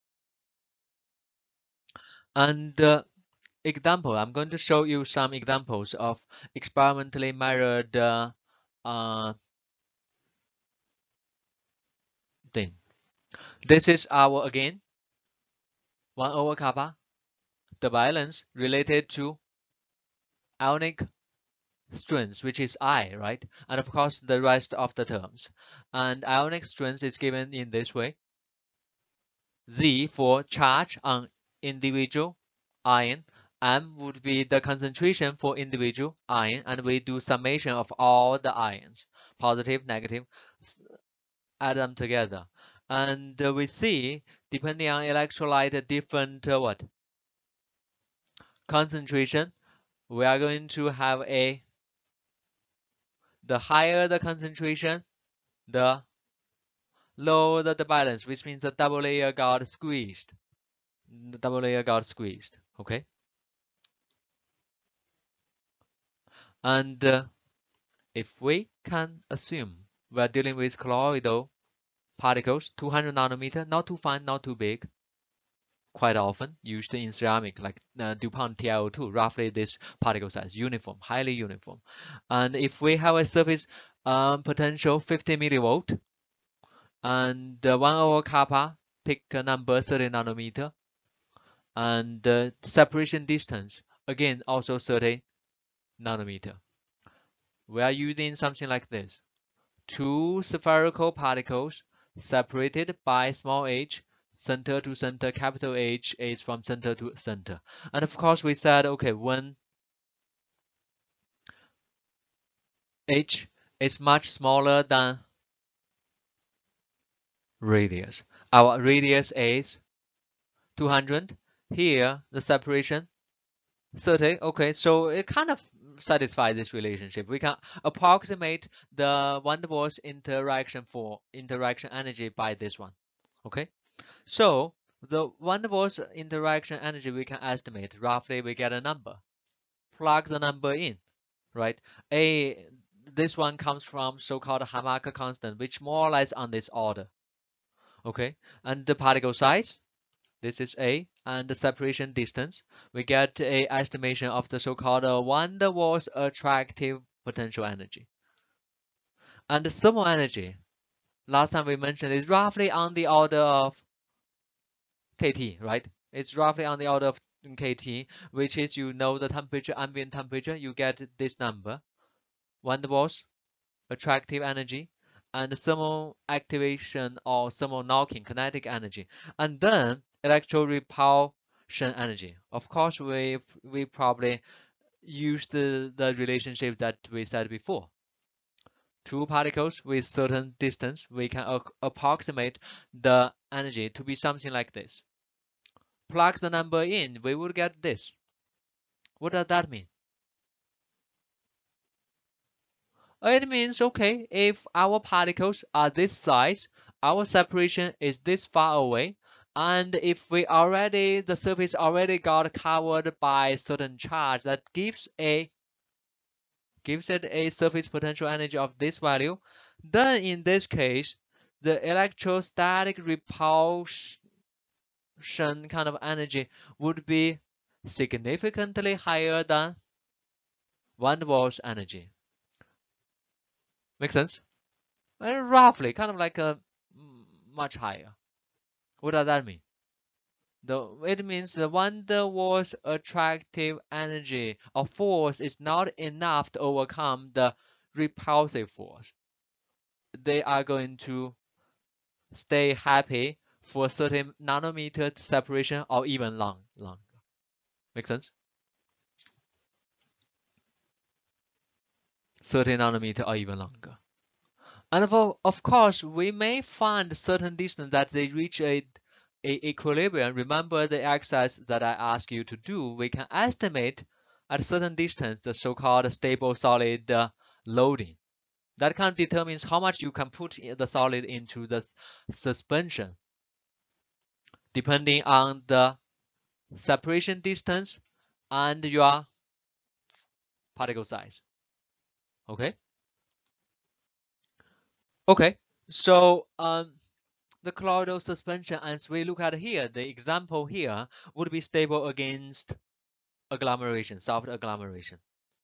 Ceramic Processing L5-10 Debye length examples
FIU EMA5646 Ceramic Processing - Lecture 5  Colloidal Processing